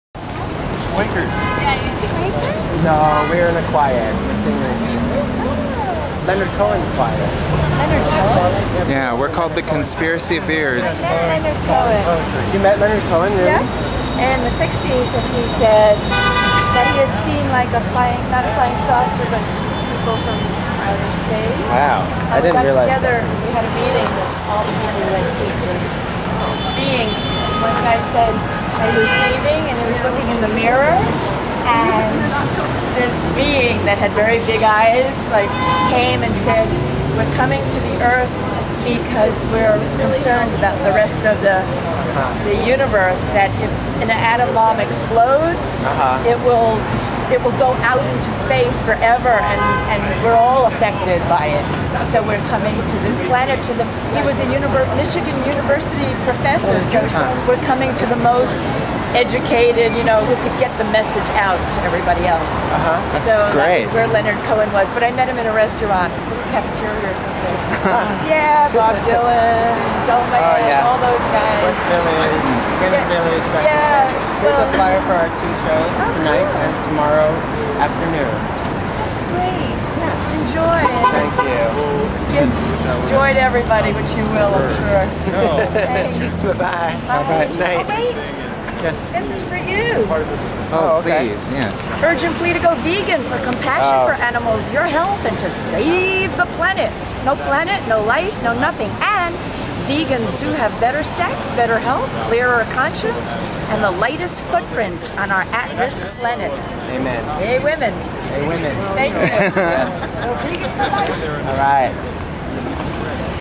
A conversation on the Bowery: Conspiracy Of Beards meet the Vegan Lady
A fun encounter: the Conspiracy Of Beards meet a great New York character with some stories to tell.